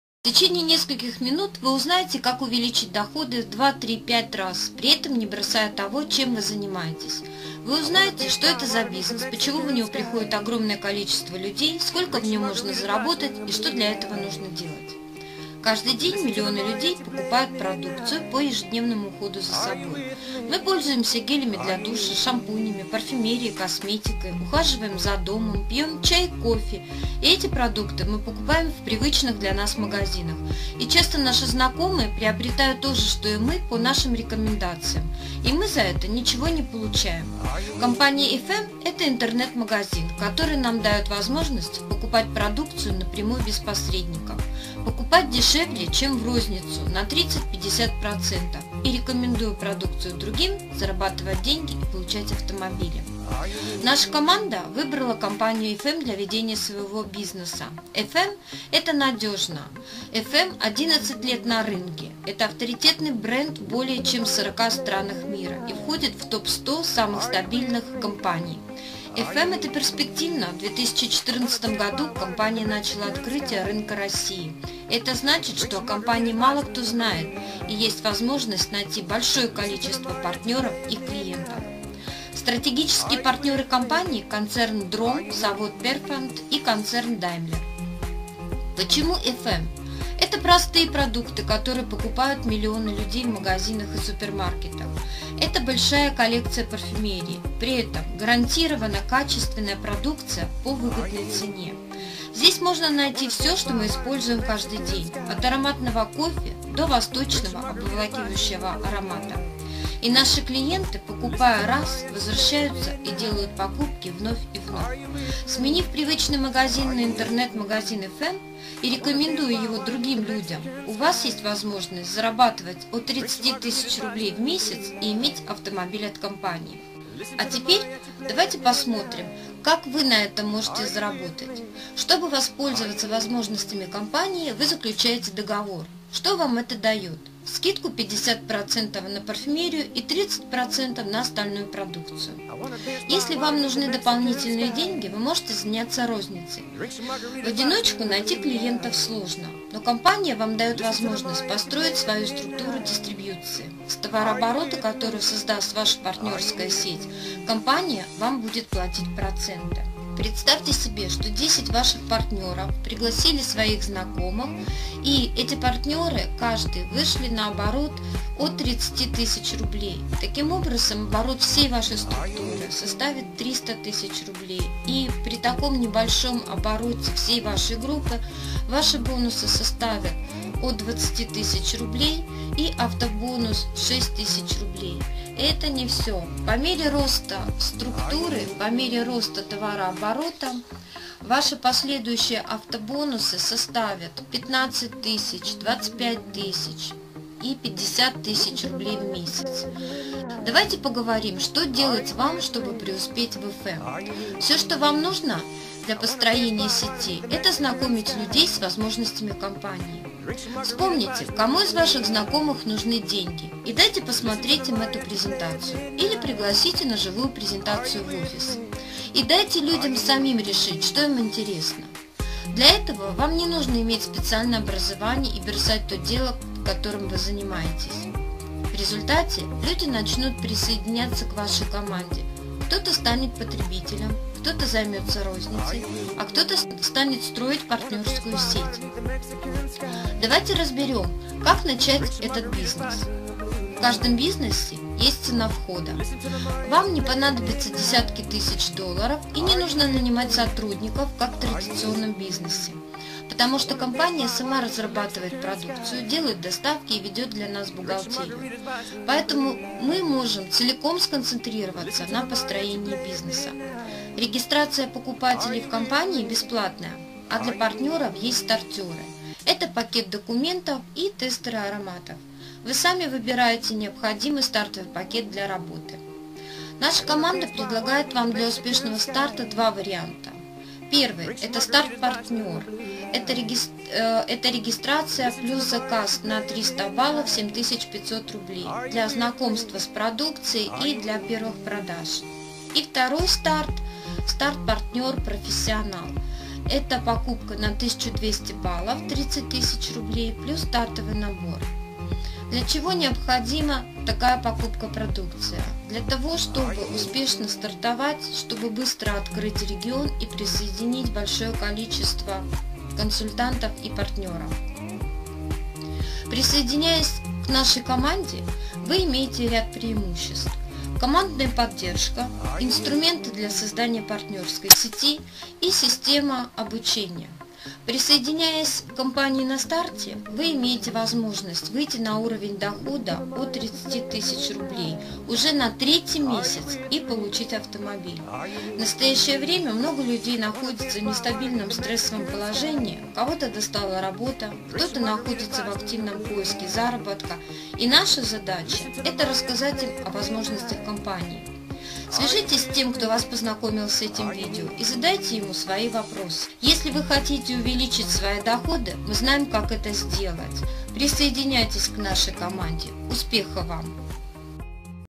FM World Russia ( FM Group Russia ) Business Presentation
This is FM World Russia (FM Group Russia)  business opportunity presentation video in Russian language.
Gediminas on:
skype: boreisaite